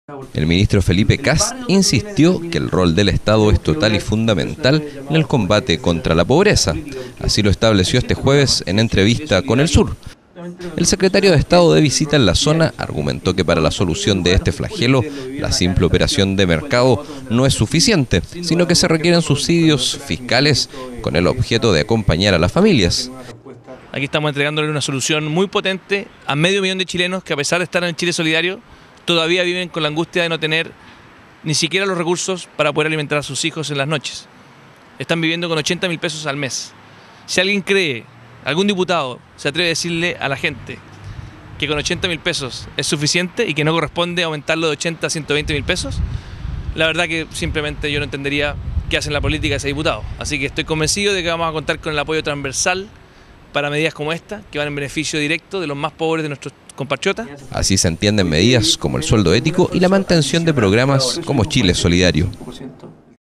Felipe Kast: Rol del Estado es clave en lucha contra la pobreza
De visita en la región, el secretario de Estado conversó con El Sur y recalcó la importancia que tiene para un país que piensa en el desarrollo avanzar con igualdad.